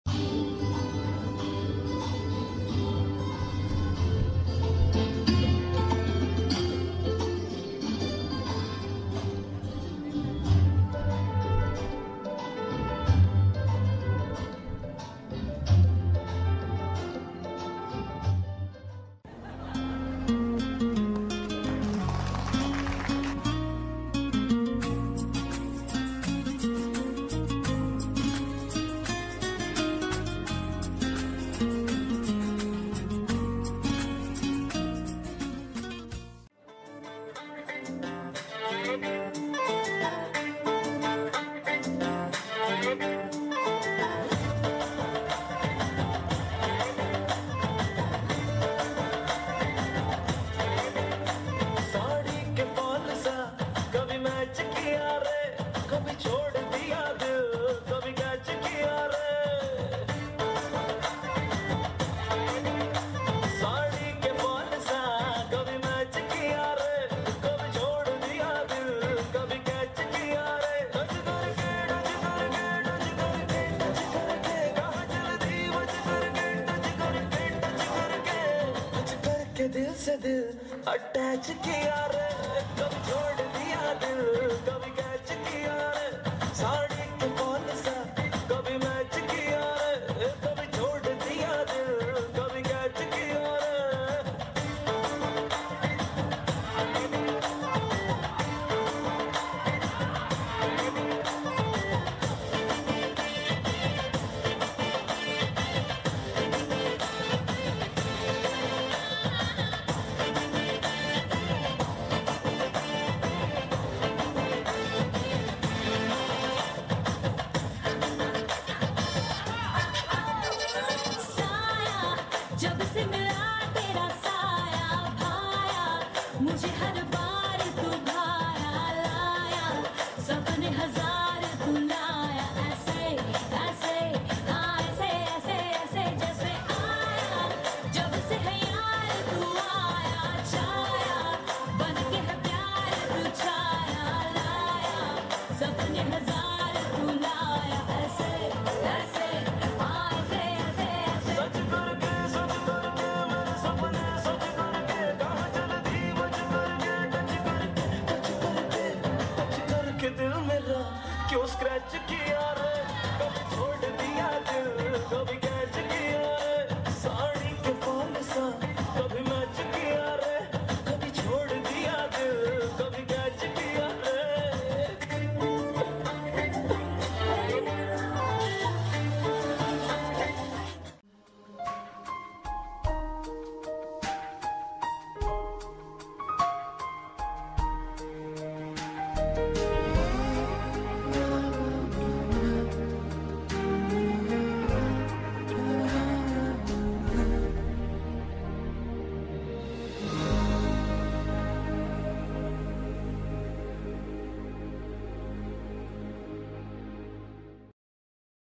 A Dance to Remember.
Memories of Druki & Timz Wedding Bash.
Filmed on Location: Water's Edge - July 27th, 2015.